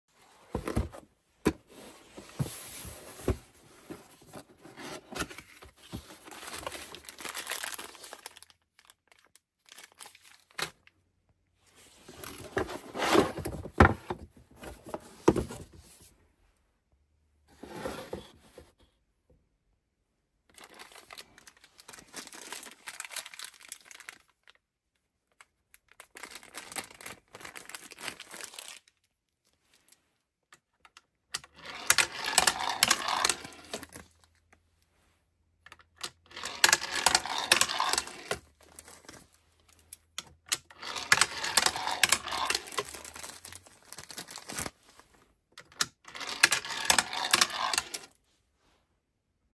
"Unveiling Wonder: Exploring the Wooden Four-Deck Pulley Toy Adventure!"
"Embark on a journey of discovery as we unbox the captivating Wooden Four-Deck Pulley Toy! Join us as we delve into the intricate craftsmanship and endless possibilities of this timeless classic. From its sturdy construction to its innovative design, each component promises to ignite curiosity and spark imagination. Whether you're a budding engineer or simply a lover of traditional toys, this video is sure to inspire awe and wonder as we unravel the magic of the four-deck pulley toy. Get ready to elevate your playtime to new heights!"